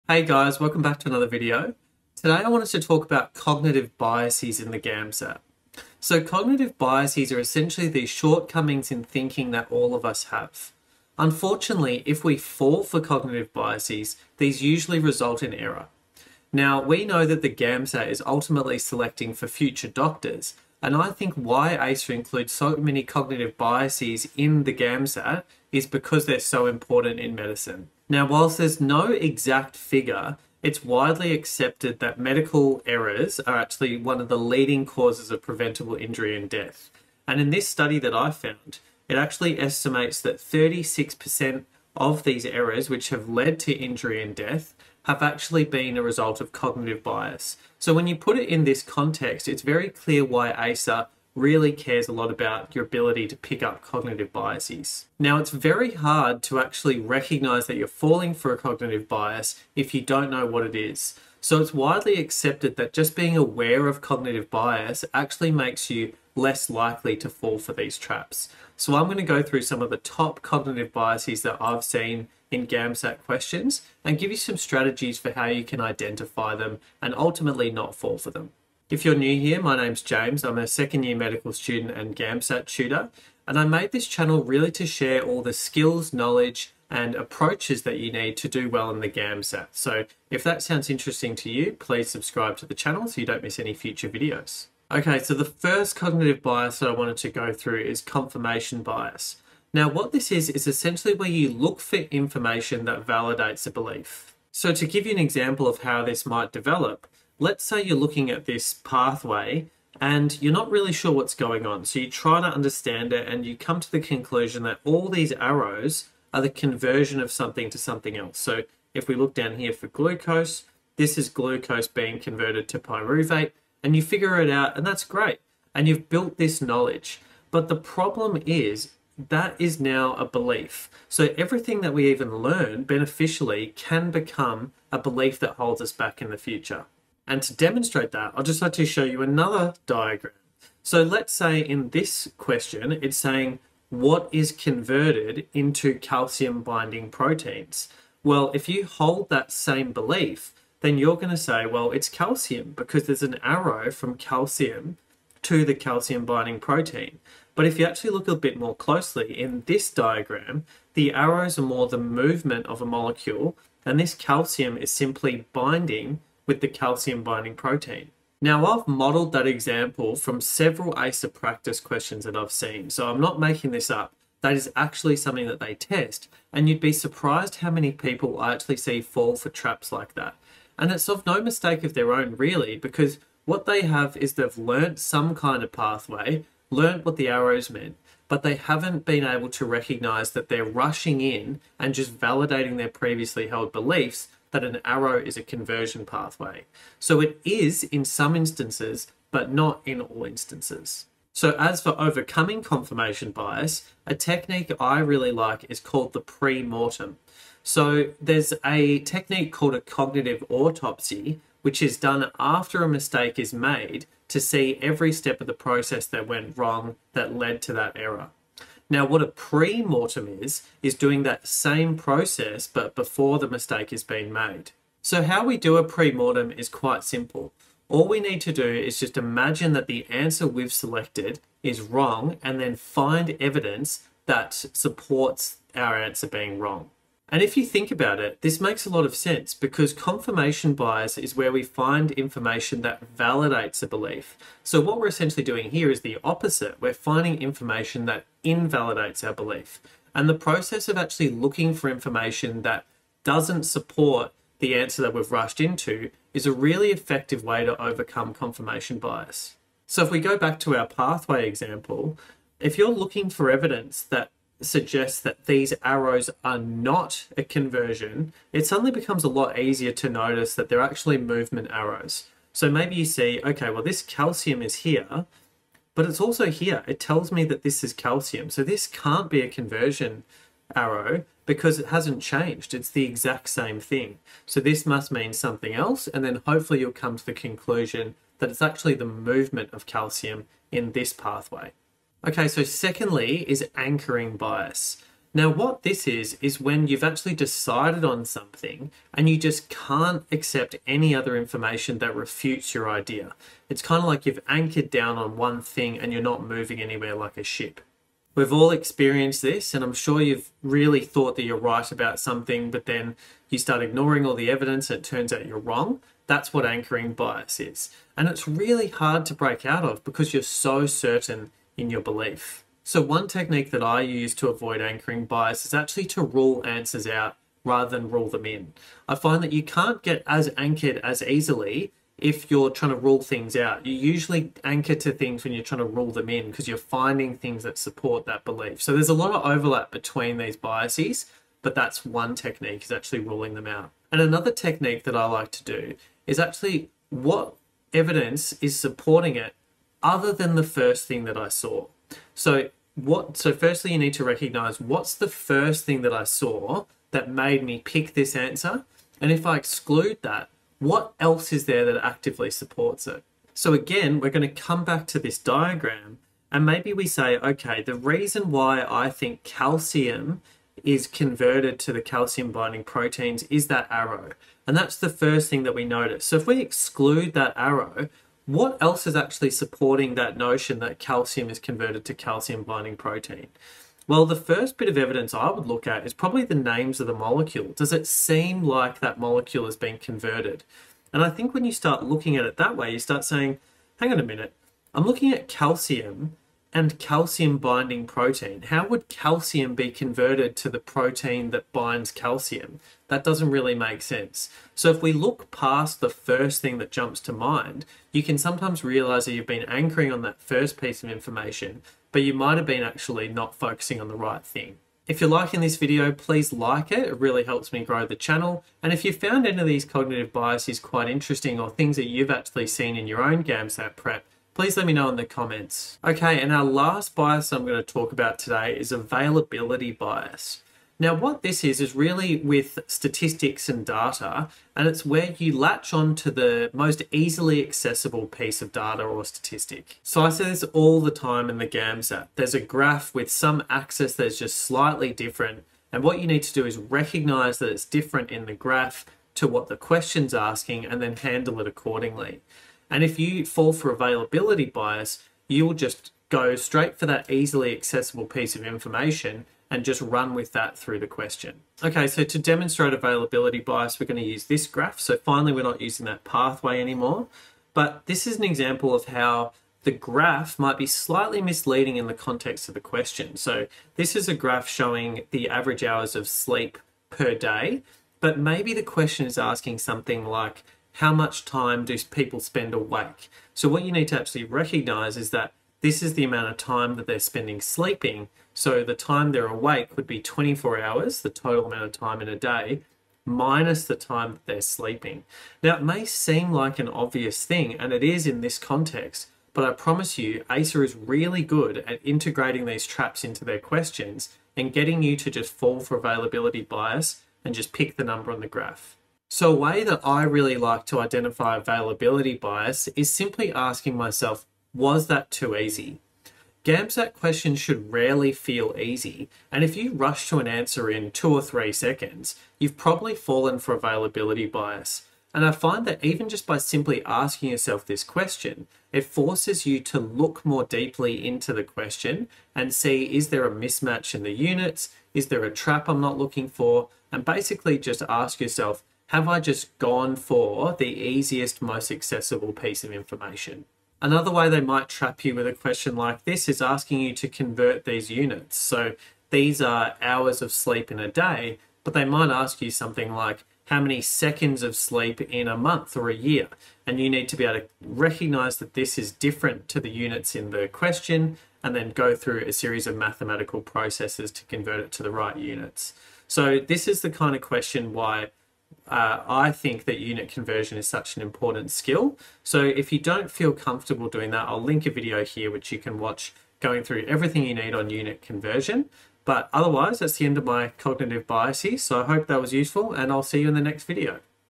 Overcoming GAMSAT Cognitive Bias Traps
Today we’re going to go through some common GAMSAT cognitive biases. I see students fall for these biases all the time and are one of the most common reasons for getting questions wrong.

Developing strategies to overcome these biases is incredibly important and one of the most important steps is to be aware of what they are. In this video I'll go through some of the most common cognitive biases I see as well as some strategies to overcome them.

Chapters
0:00 Why are cognitive biases important in the GAMSAT?
1:35 Confirmation bias
4:52 Anchoring bias
7:06 Availability bias